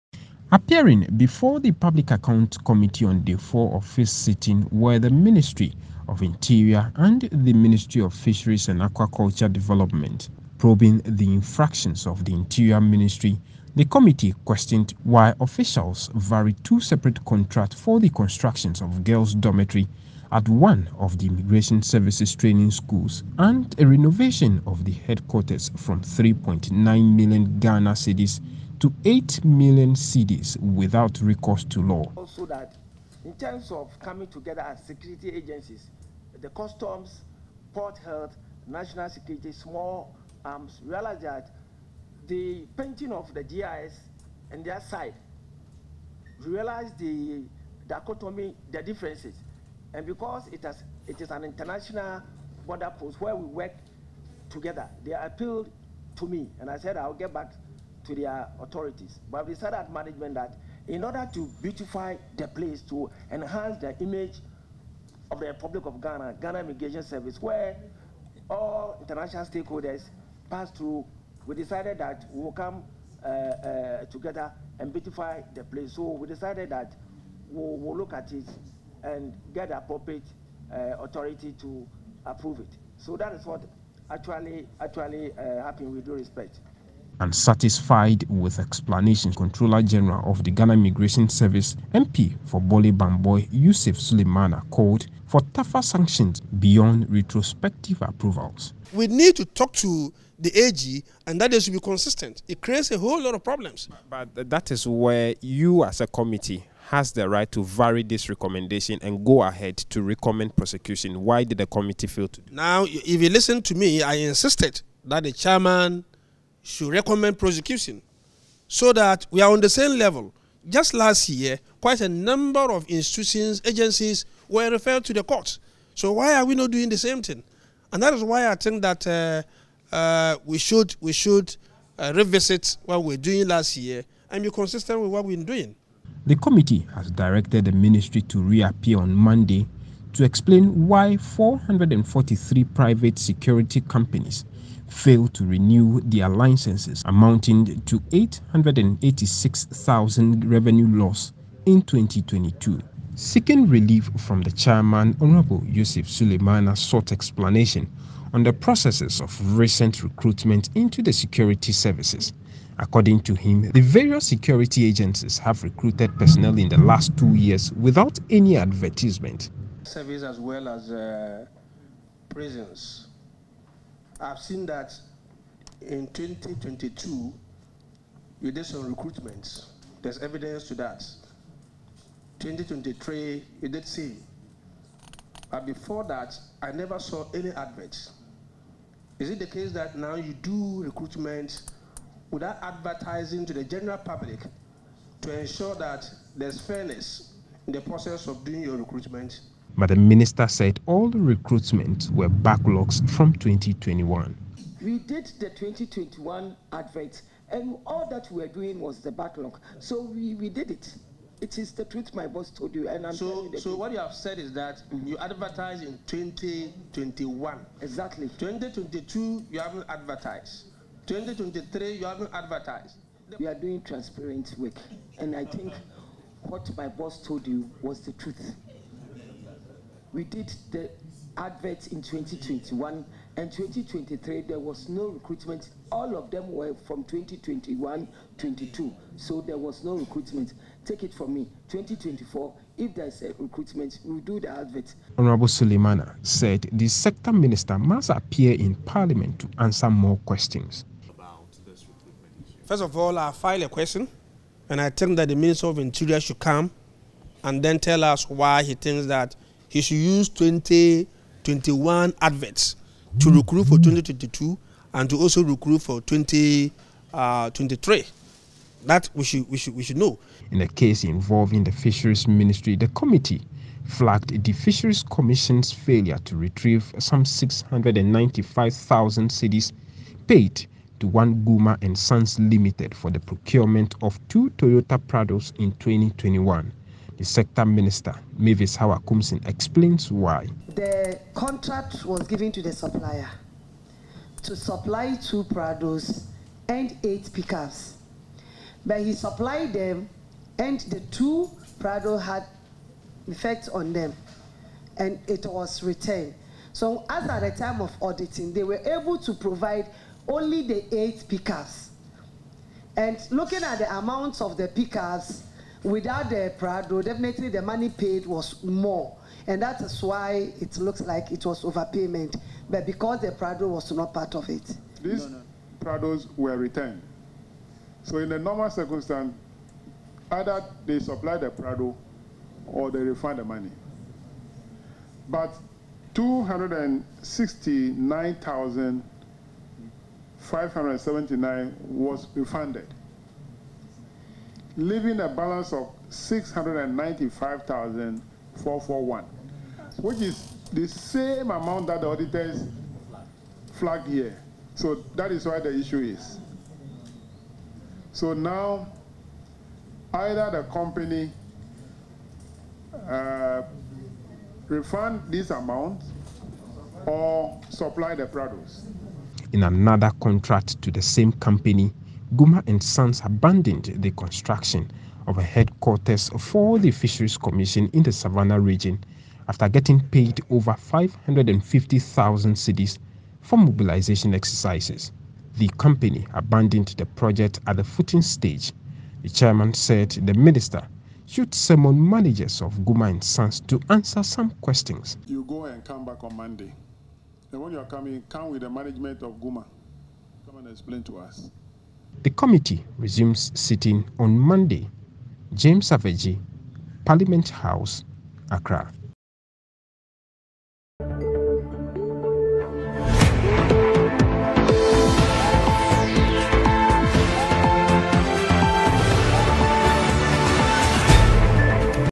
PAC: Interior Minister must explain recent unadvertised recruitment processes - Yusif Sulemana
Public Accounts Committee sitting: Interior Minister must explain recent unadvertised recruitments processes. - Yusif Sulemana